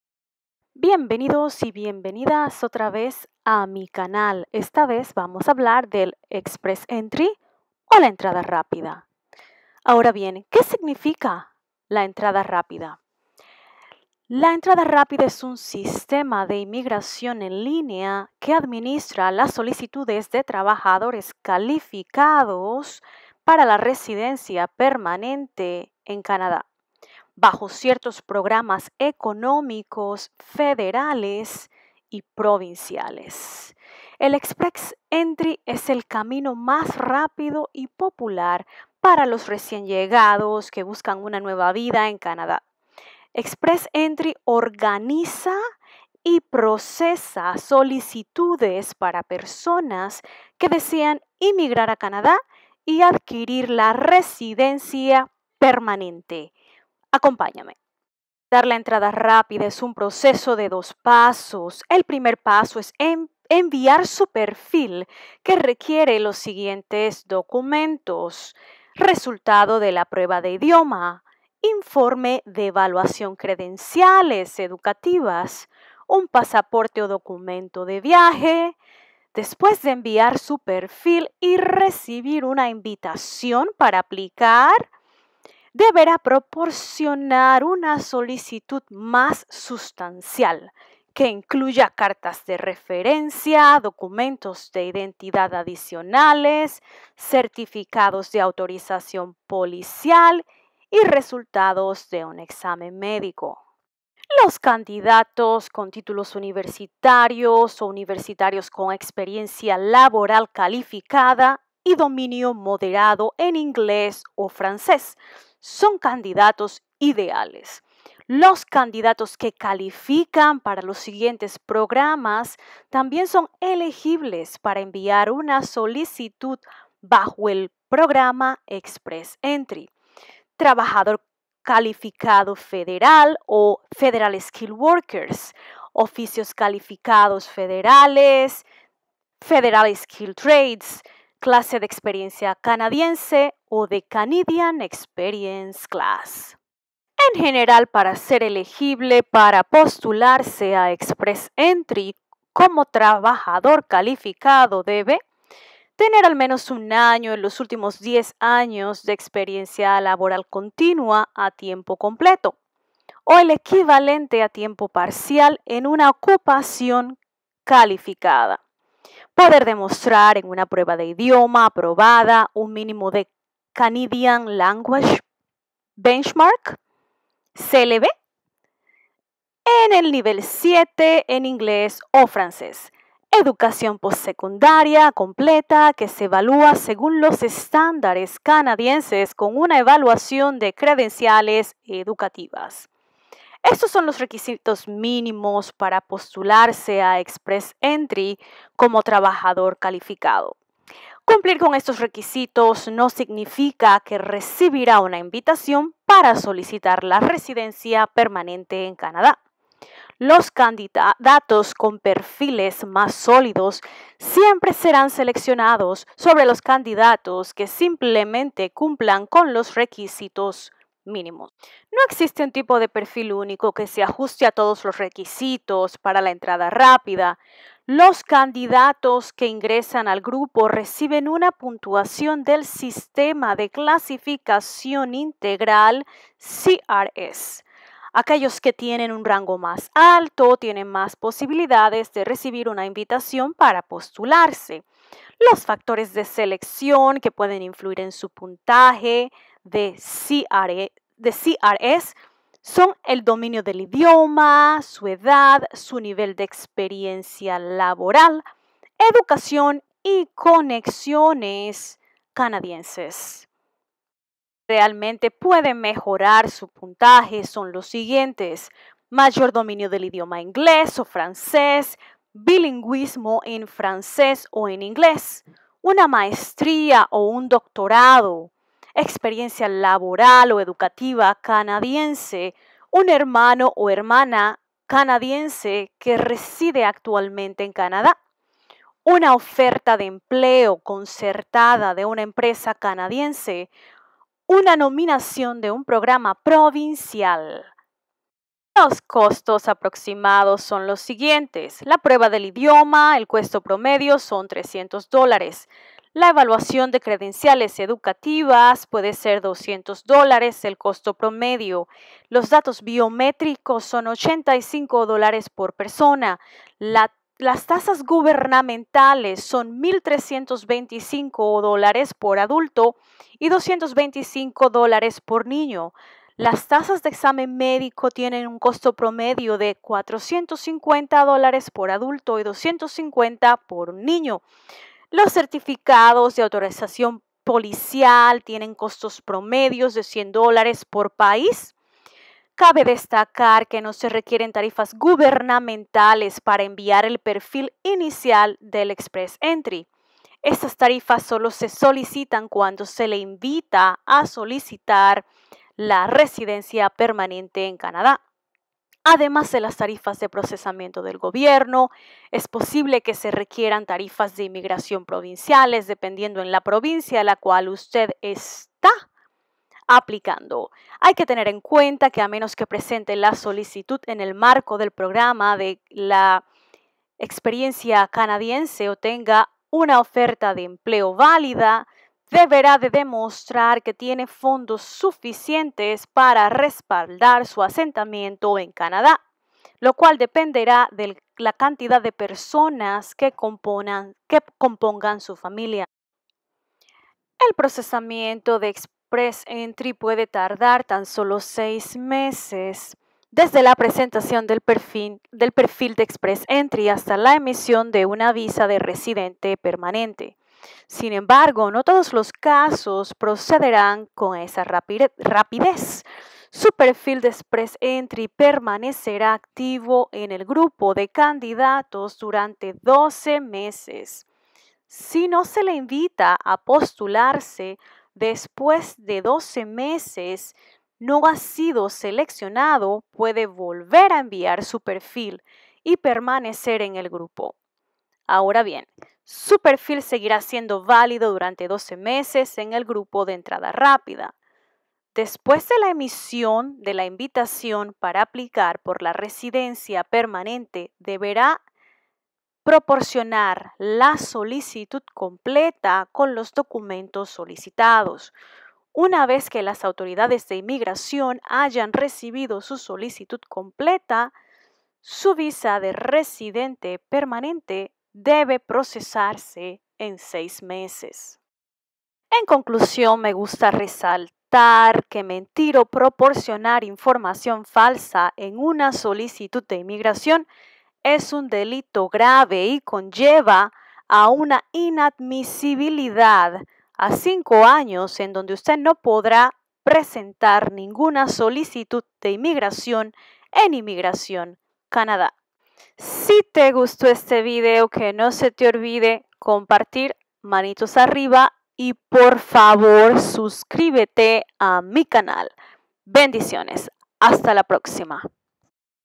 Express Entry
En el video de hoy hablaremos.

Express Entry es un sistema de inmigración en línea que administra las solicitudes de trabajadores calificados para la residencia permanente en Canadá bajo ciertos programas económicos federales y provinciales.

Es el camino más rápido y popular para los recién llegados que buscan una nueva vida en Canadá.

Express Entry organiza y procesa solicitudes para personas que desean inmigrar a Canadá y adquirir la residencia permanente canadiense.